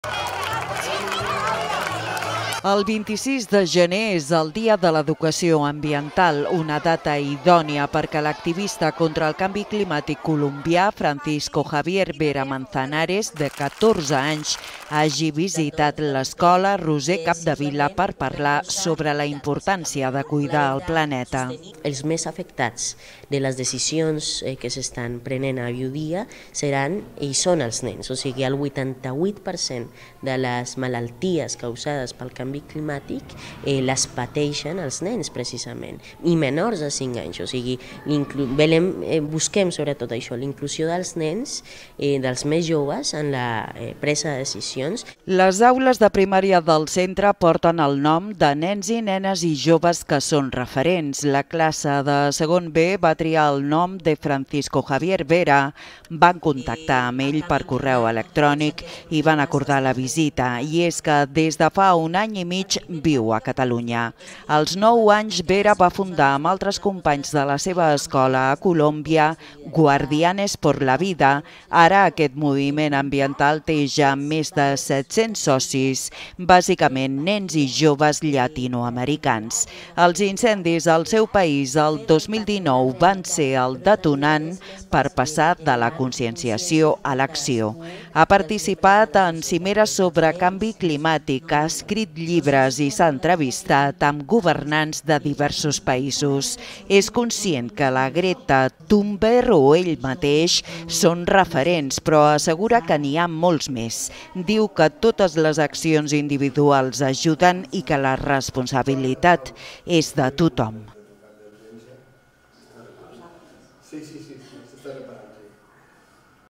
Francisco Javier Vera: ''Busquem la inclusió dels nens, nenes i joves en la presa de decisions"
El 26 de gener és el Dia de l’Educació Ambiental. Una data idònia perquè l’activista contra el canvi climàtic colombià, Francisco Javier Vera Manzanares, de 14 anys, hagi visitat l’Escola Roser Capdevila per parlar sobre la importància de cuidar el planeta.

Les aules de primària del centre porten el nom de nens i nenes i joves que són referents. La classe de 2n B va triar el nom de Francisco Javier Vera, van contactar amb ell per correu electrònic i van acordar la visita. I és que, des de fa un any i mig viu a Catalunya.

Als 9 anys, Vera va fundar amb altres companys de la seva escola a Colòmbia «Guardianes por la vida». Ara aquest moviment ambiental té ja més 700 socis, bàsicament nens i joves llatinoamericans.

Els incendis al seu país el 2019 van ser el detonant per passar de la conscienciació a l’acció.

Ha participat en cimeres sobre canvi climàtic, ha escrit llibres i i s’ha entrevistat amb governants de diferents països.

És conscient que la Greta Thun berg o ell mateix són referents però assegura que n’hi ha molts més. Diu que totes les accions individuals ajuden i que la responsabilitat és de tothom.